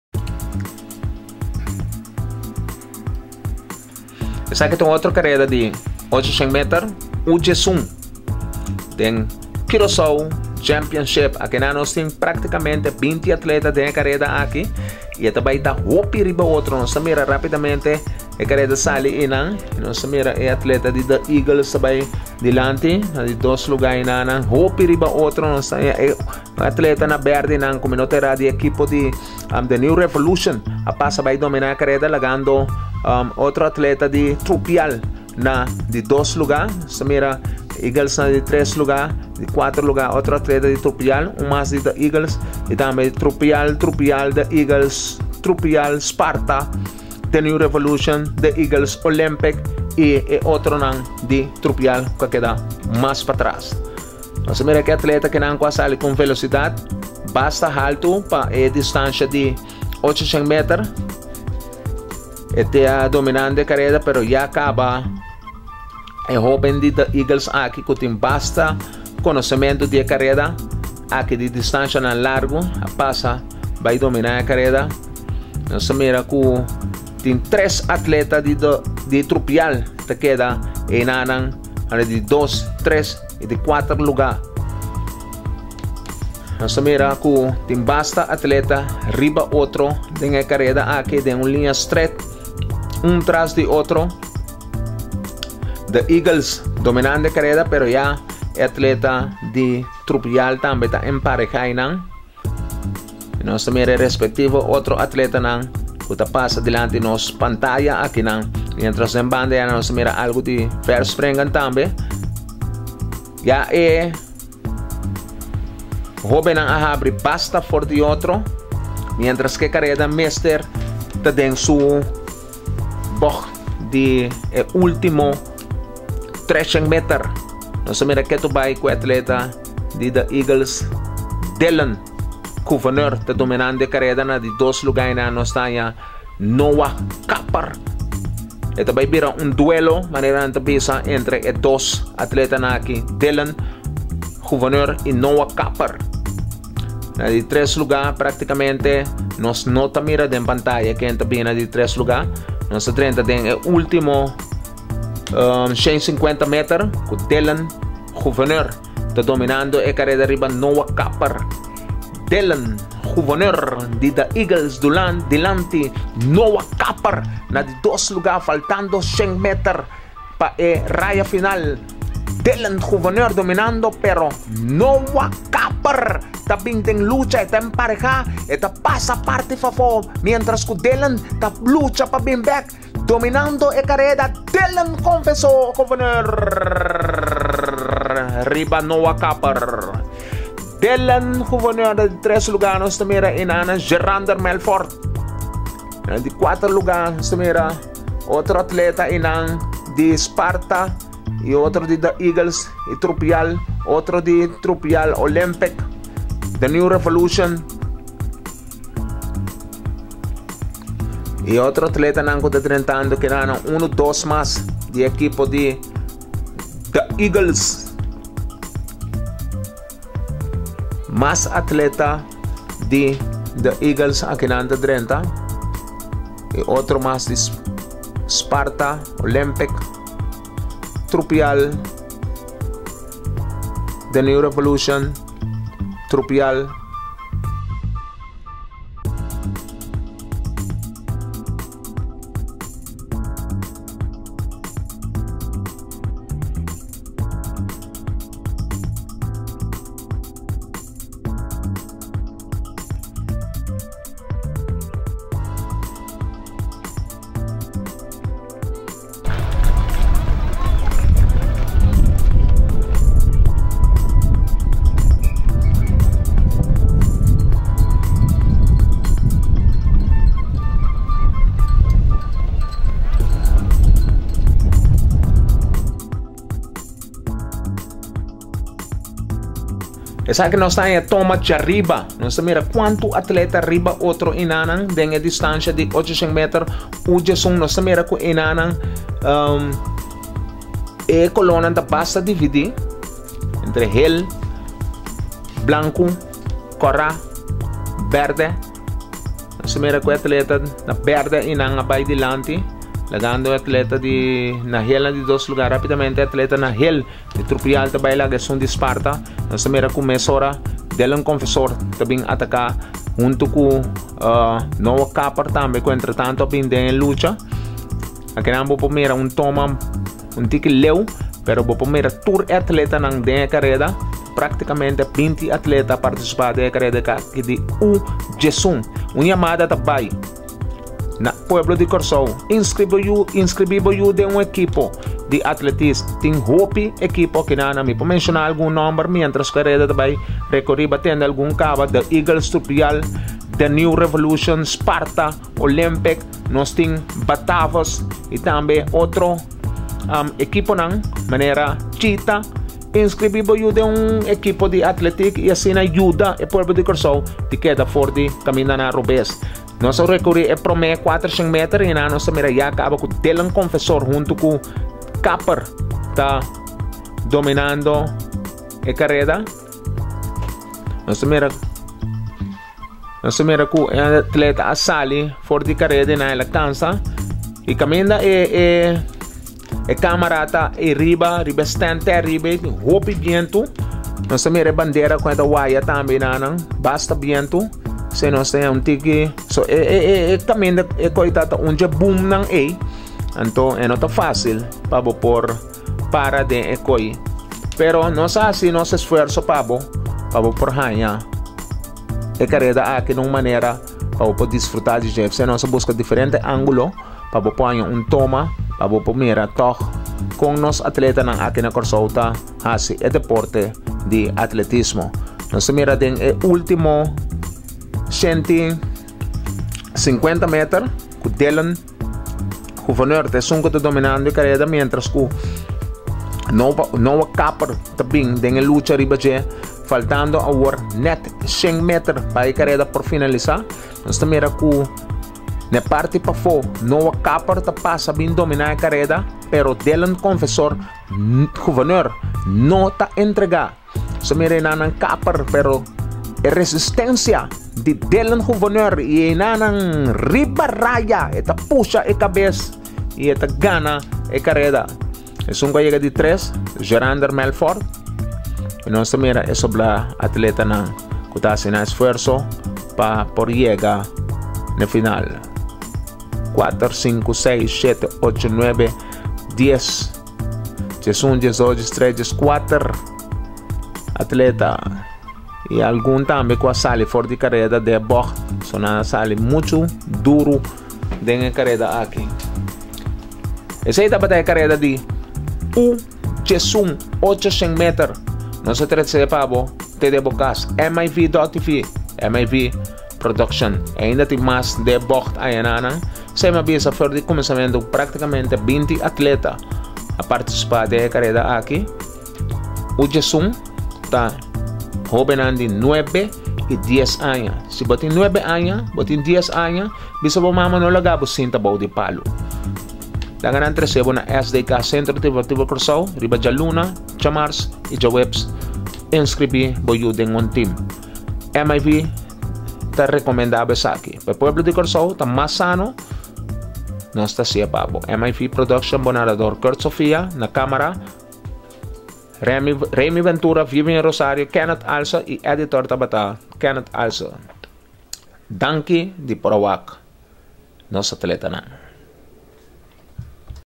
Athletics Curacao National Championship 800m Boys U 11 21 06 2014 by miv.tv curacao
By miv productions for miv.tv curaçao